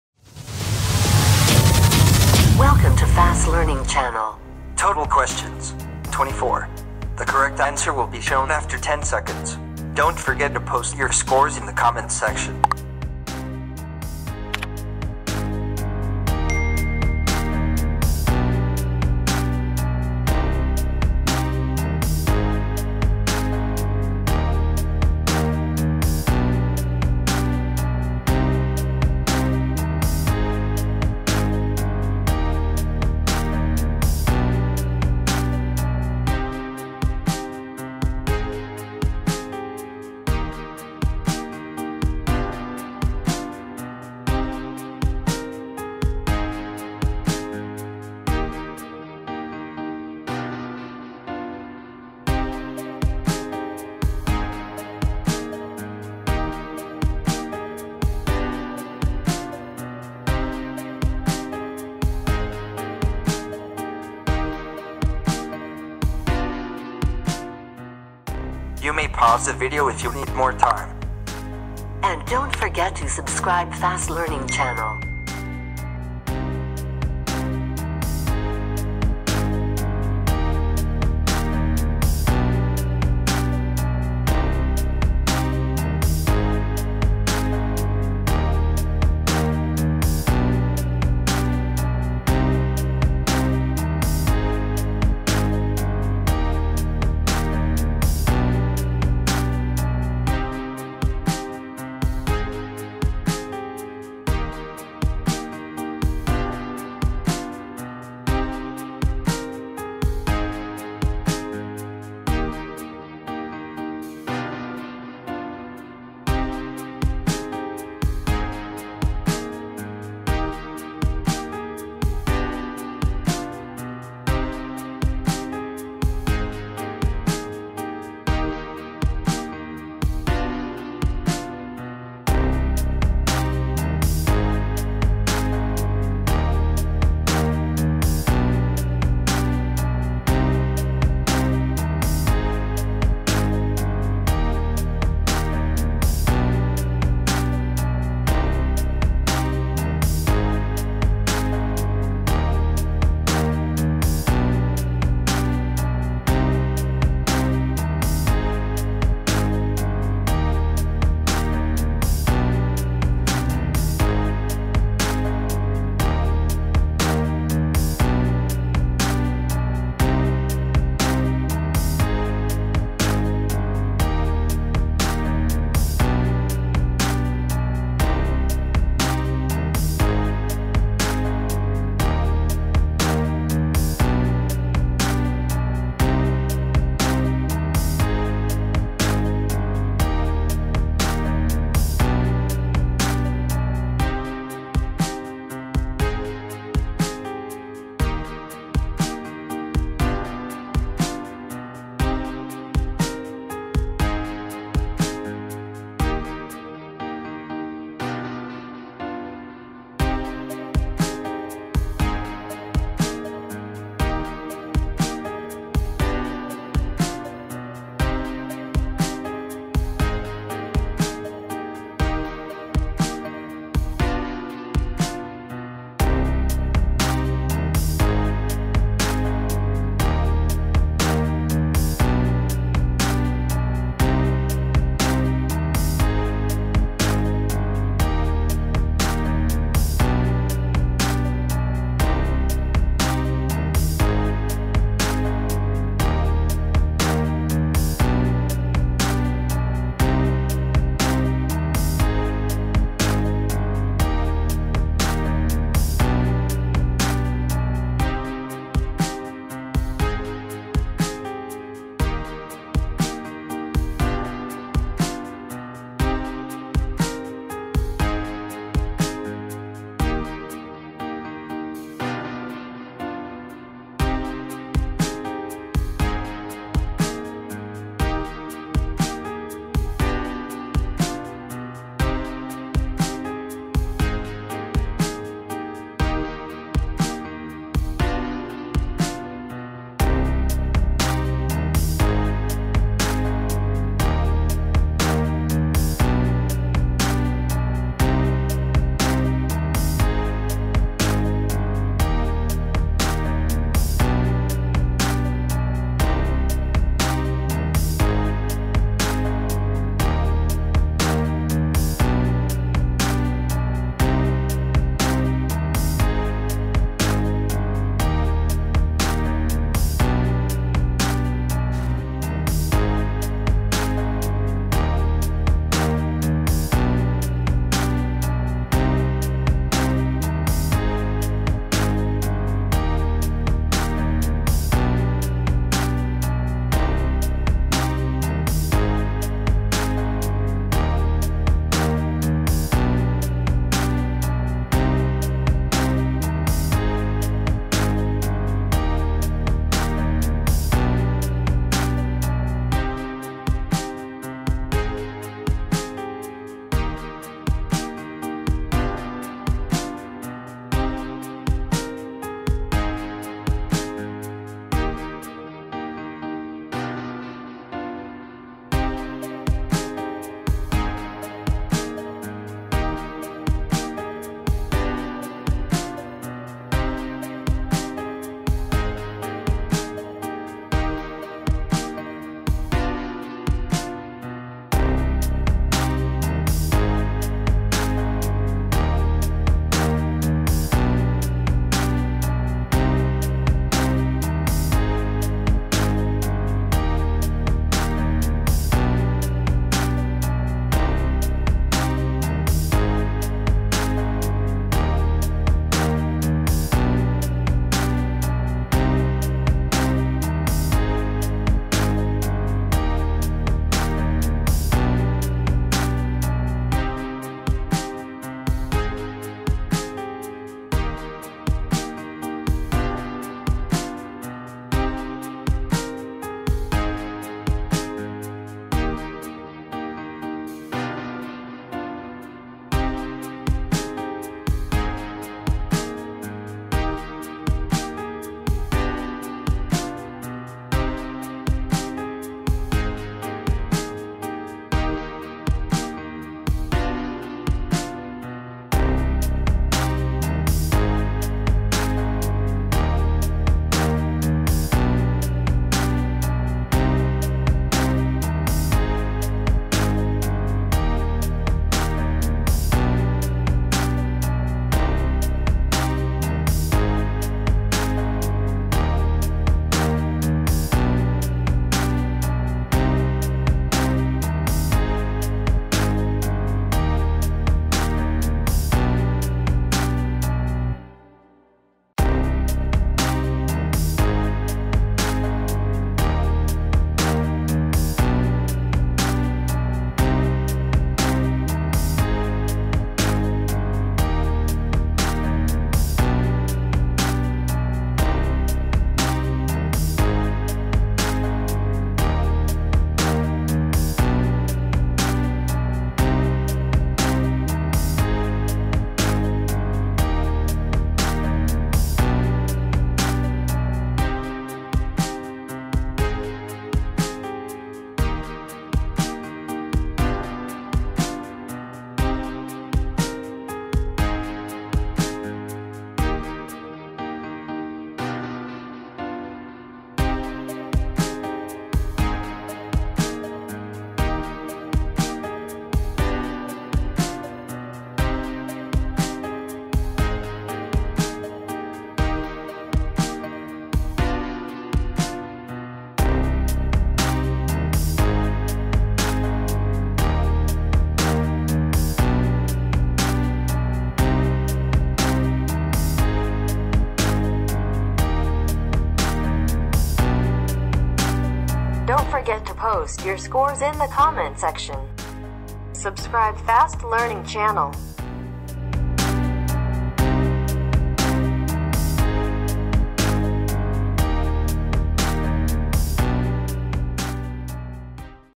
Active and Passive Grammar Exercise Quizzes & Test | Comparison Of Adjectives | Mixed Grammar Test
The Correct Answer Will Be Shown After 10 Seconds.

Don't Forget to Post Your Scores in the Comment Section